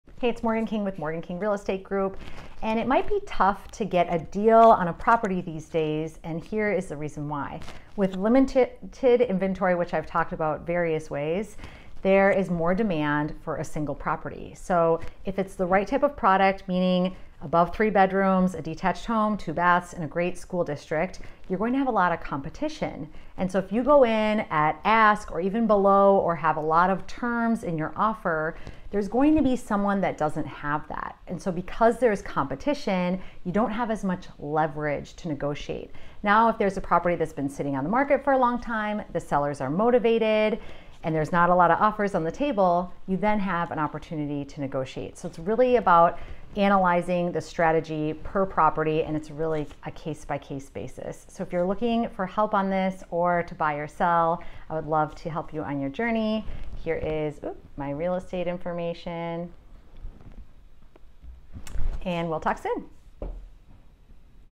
Why Getting a Deal is Less Likely in this Market
While I firmly believe that what is meant to be yours cannot pass you by, I am also aware that getting a deal when buying a property in the SoCal market is happening less and less these days. To learn why this is happening, checkout my latest video above.